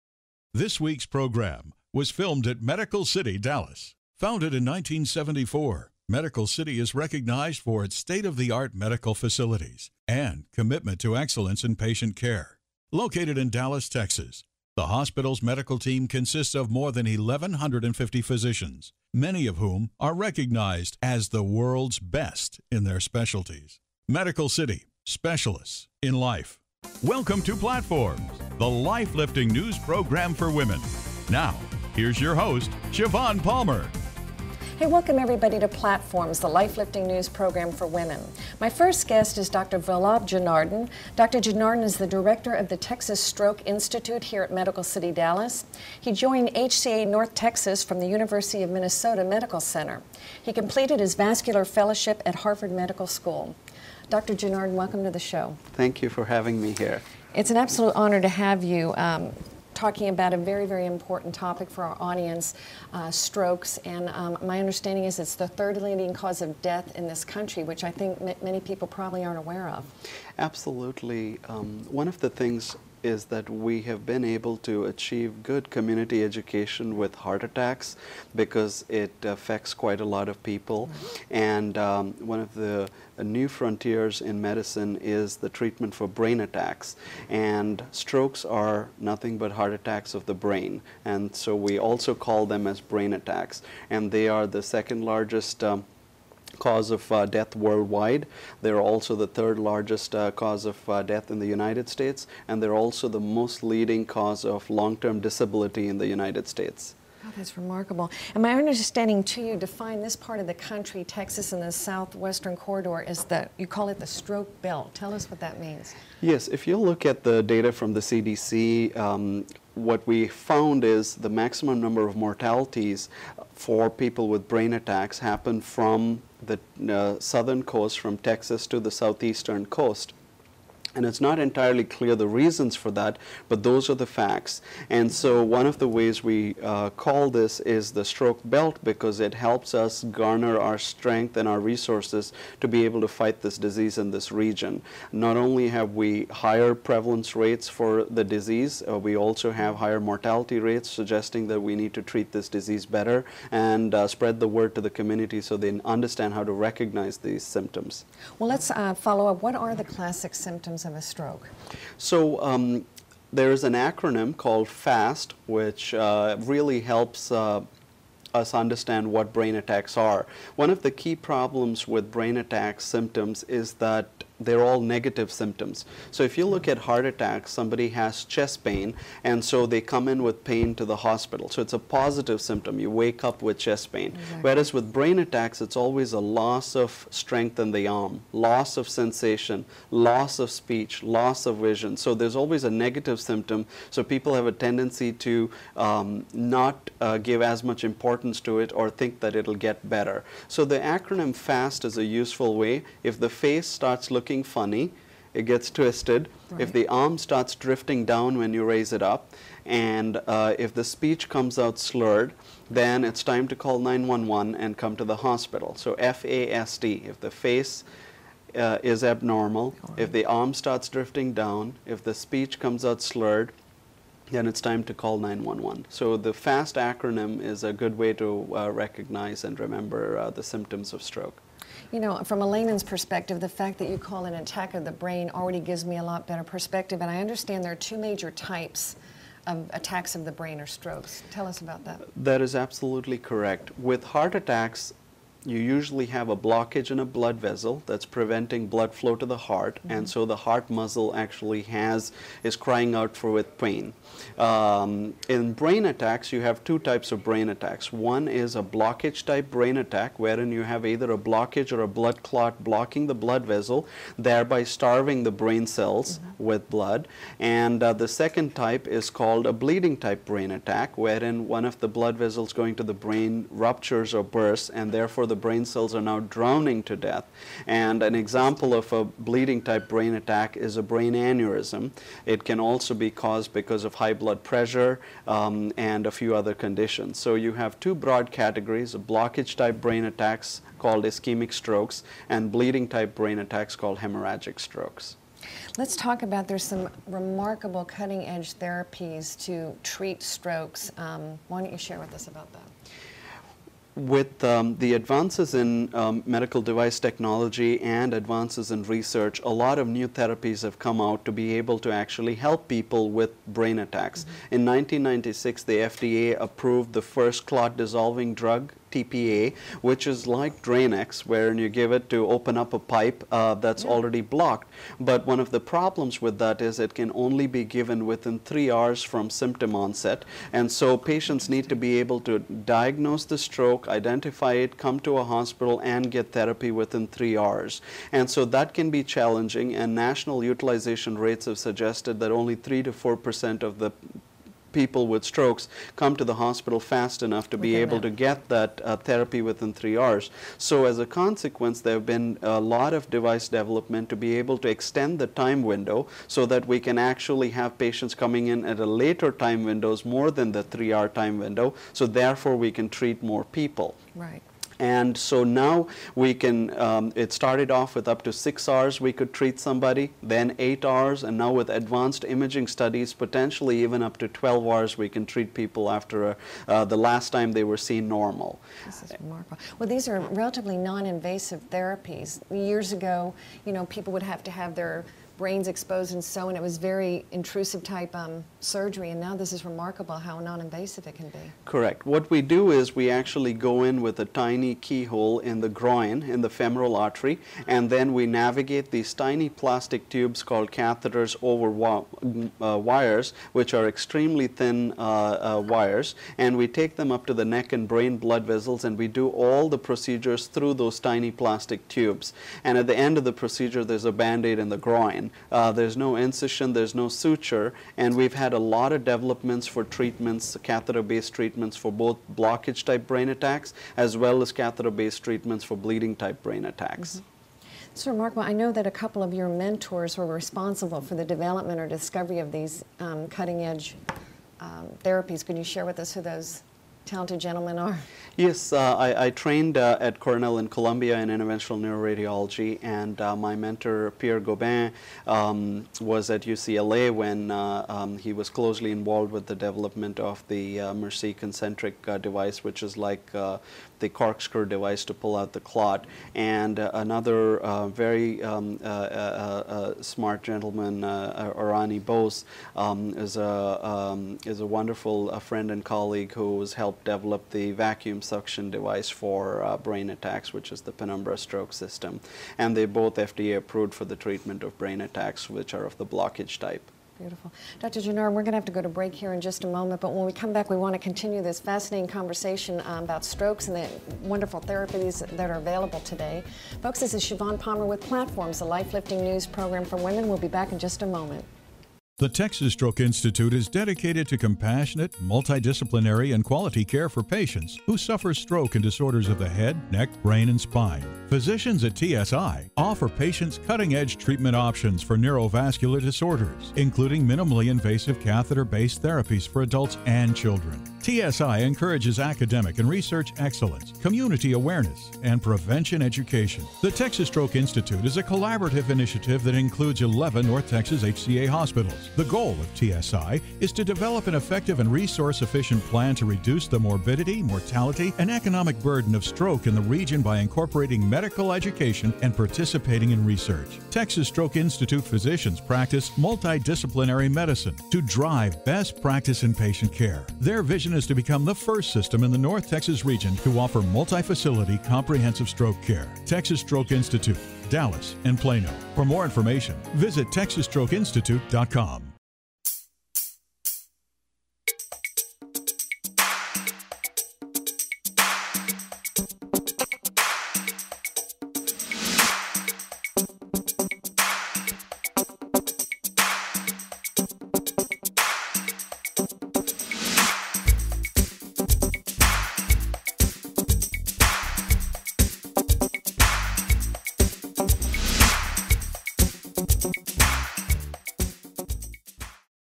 Healthcare Interviews: Dr. Vallabh Janardhan, MD
Dr. Janardhan joined HCA North Texas from the University of Minnesota Medical Center, where he served as Medical Director for Stroke and Interventional Neuroradiology. He completed his Vascular Fellowship at Harvard Medical School and his Interventional Neuroradiology Fellowship at Cornell and Columbia Universities.  Dr. Janardhan's areas of expertise include interventional neuroradiology, cutting edge therapies for stroke treatment, and neuro critical care. He is a founding member of the Society of Vascular and Interventional Neurology and currently serves on their Board of Directors.